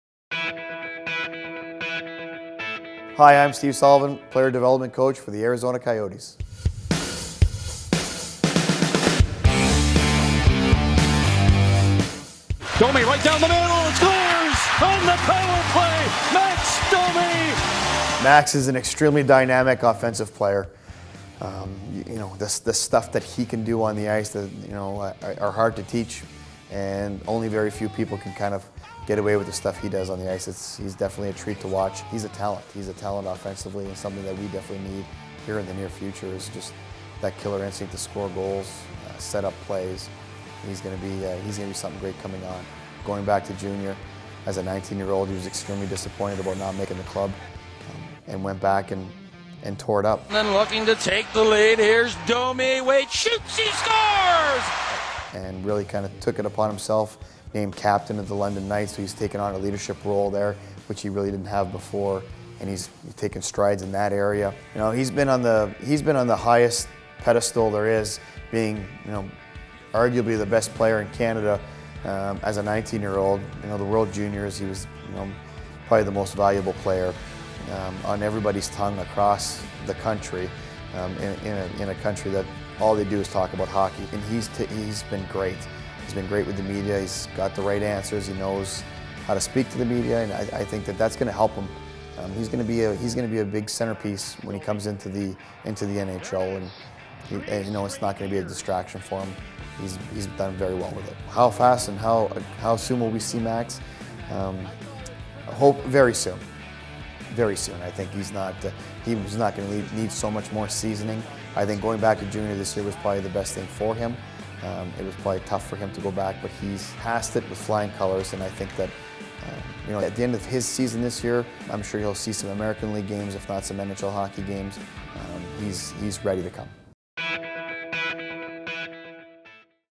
Young Guns: Max Domi
Take an inside look at the future of the Coyotes with Player Development Coach, Steve Sullivan.